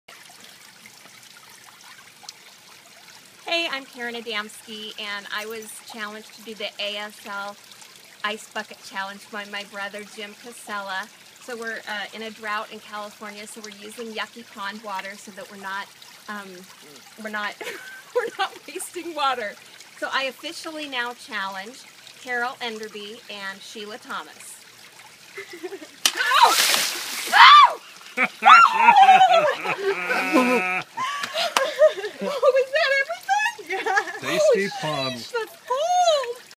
ALS Ice Bucket Challenge - Karen Adamski
My brother, Jim Casella, challenged me tot he ALS Ice Bucket Challenge.  In the video, I say "ASL" - oops, sorry.  I now challenge Carole Enderby and Sheila Thomas.